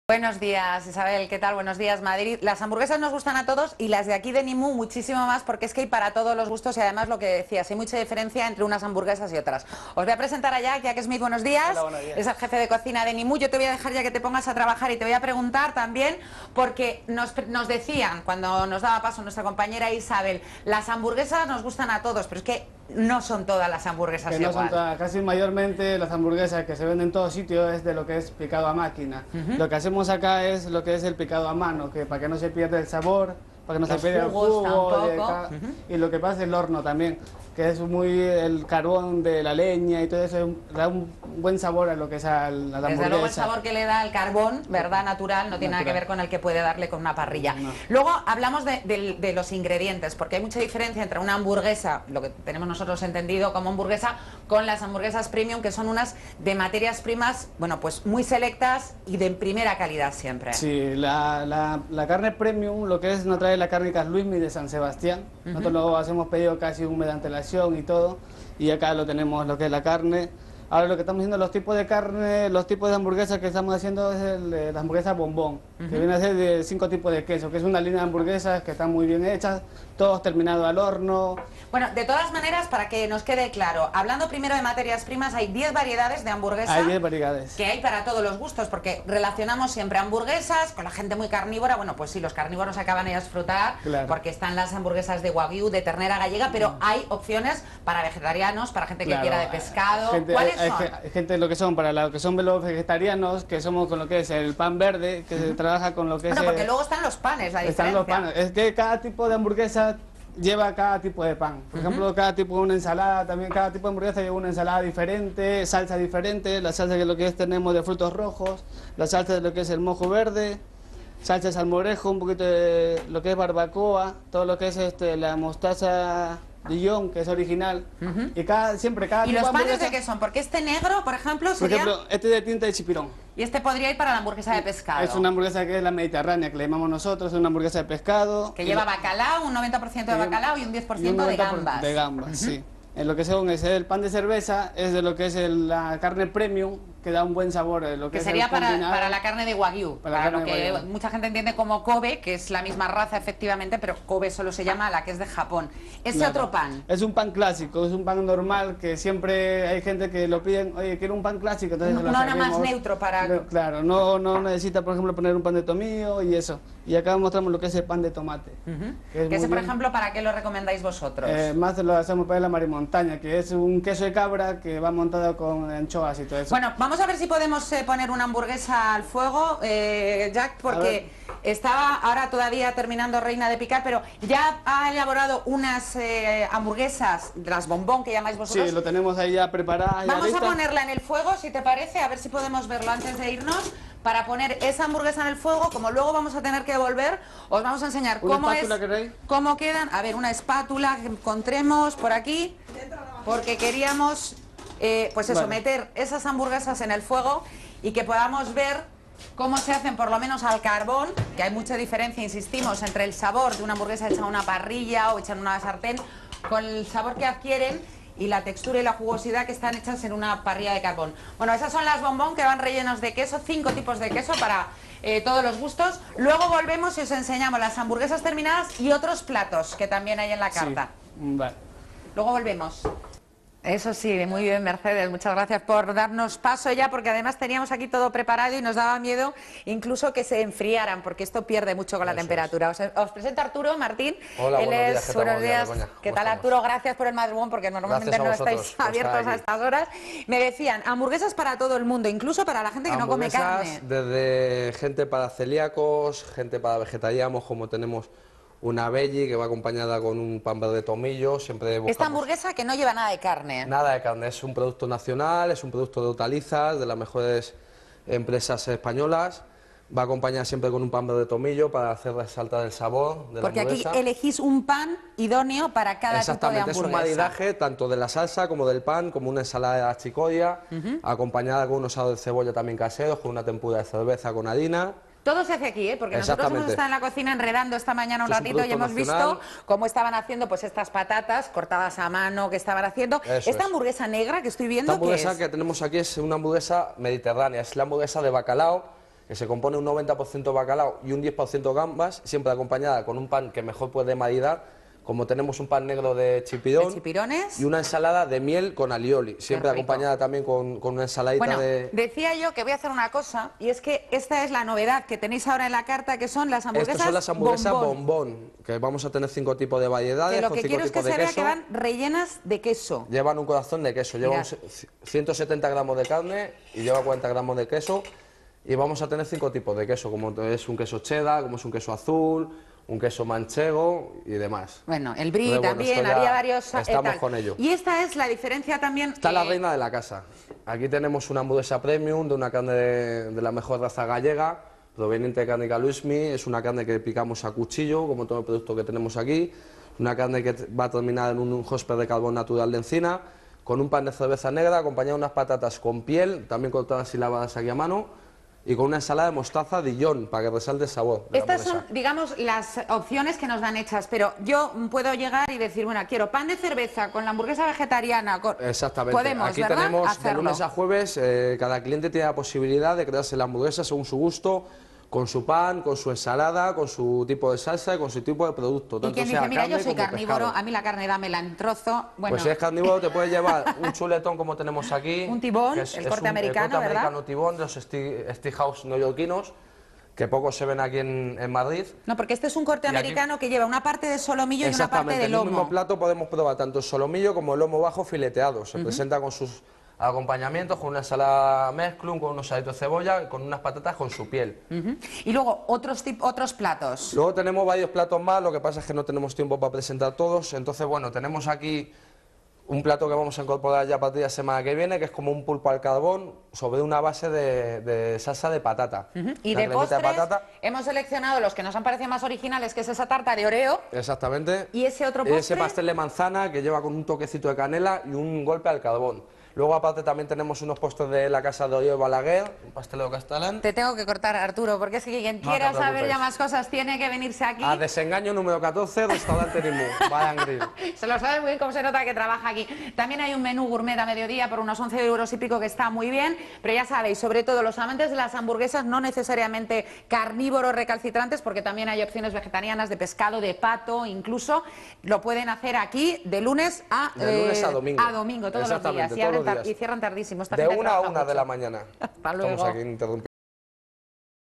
Hamburguesas premium
Es el plato de moda. Las hamburguesas 'premium' se han hecho rápidamente un hueco en el panorama gastronómico por sobrados motivos: se elaboran con materias primas de primera calidad, son una opción relativamente asequible y, sobre todo, son deliciosas.